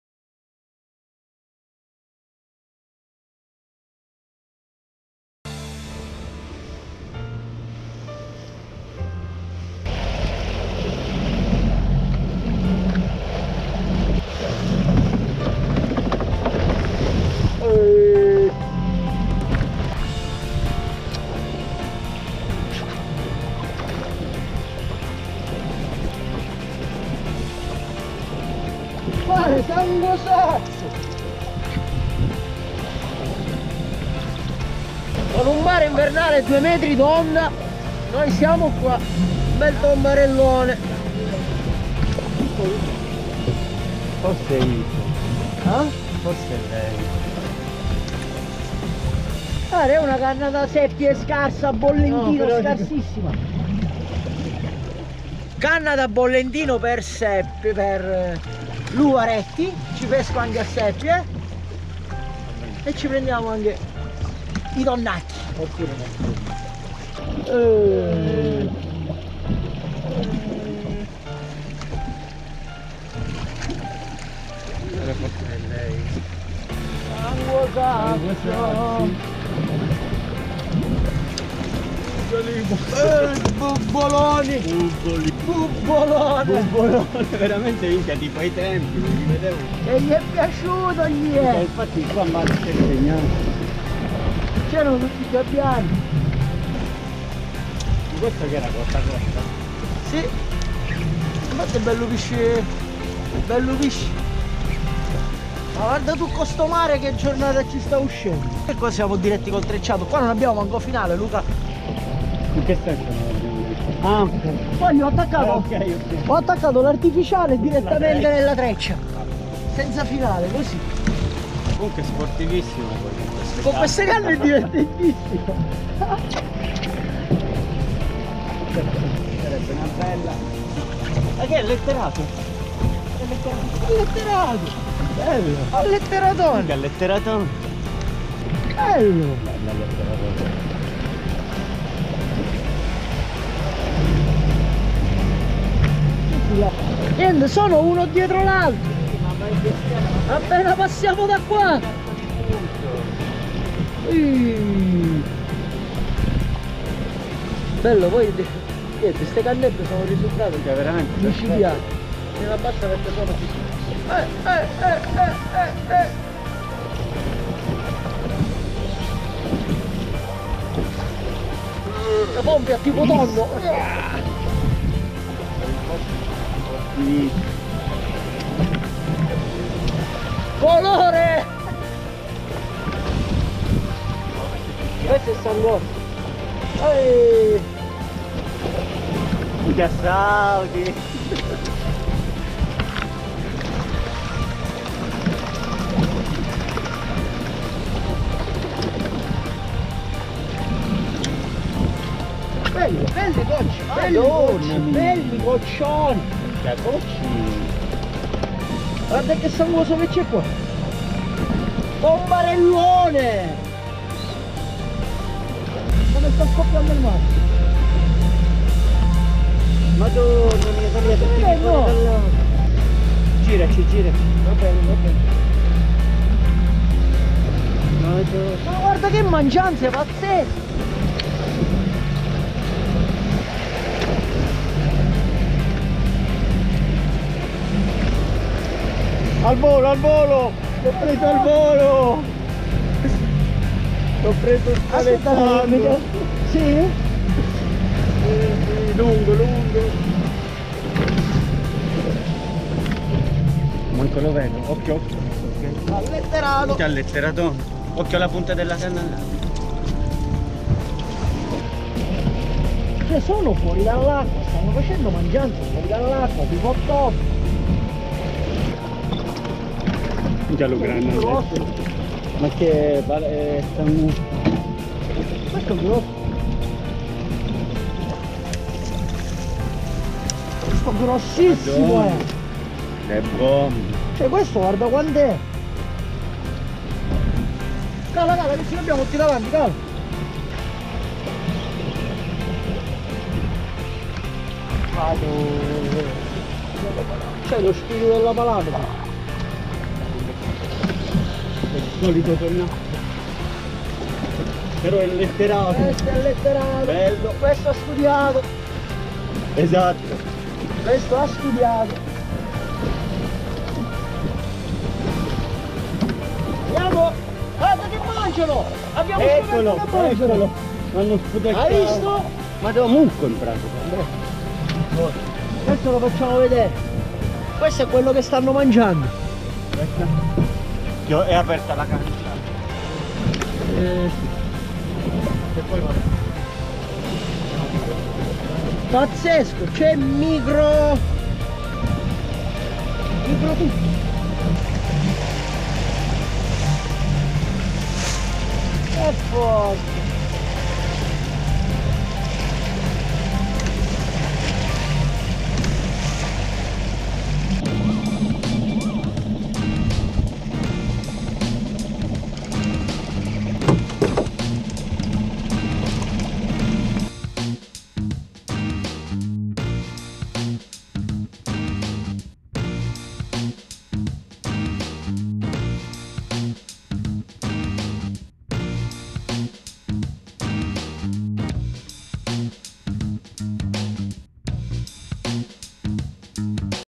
Tonni di branco invernali con mare molto mosso - Pesca a traina con artificiali
Una mattina di pesca con mare molto agitato,. Notiamo che il mare brulica di vita in superficie, e decidiamo di pescare a traina con gli artificiali. Le catture si susseguono tra tombarelli e alletterati, nonostante il mare formato. Una bella mattinata divertente